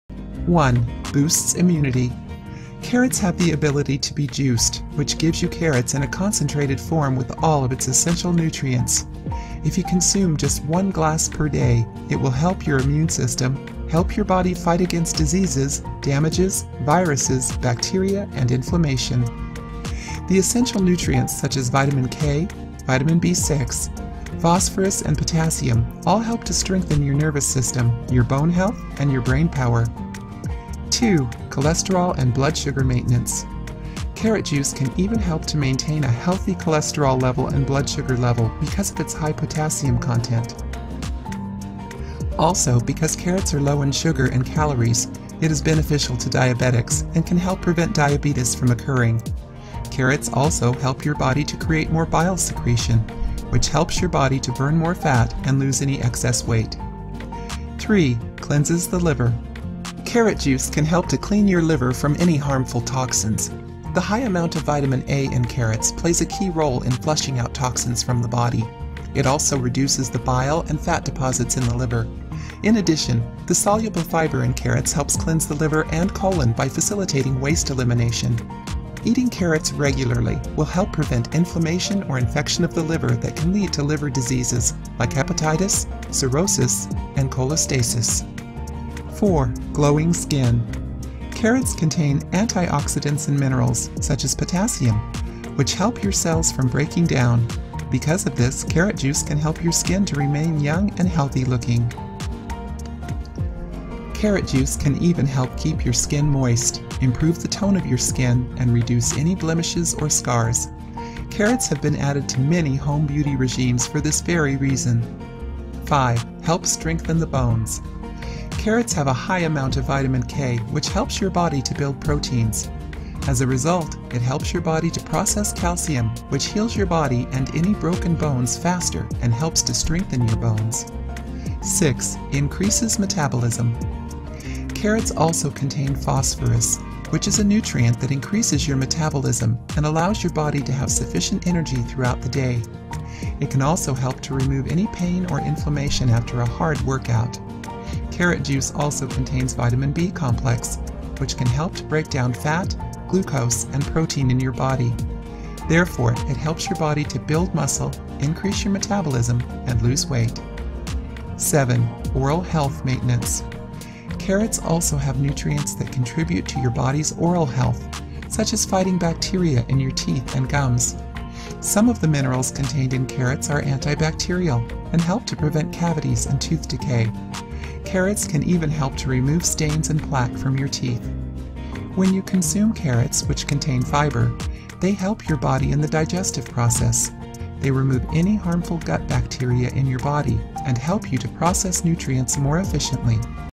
Clean Your Arteries: What happens if you eat carrots everyday? What are the benefits ofeatingcarrots
Clean Your Arteries: What happens if you eat carrots everyday? What are the benefits of eating carrots

Therefore, if you have any questions or doubts, you should consult your doctor. It is extremely important that you act responsibly in this regard.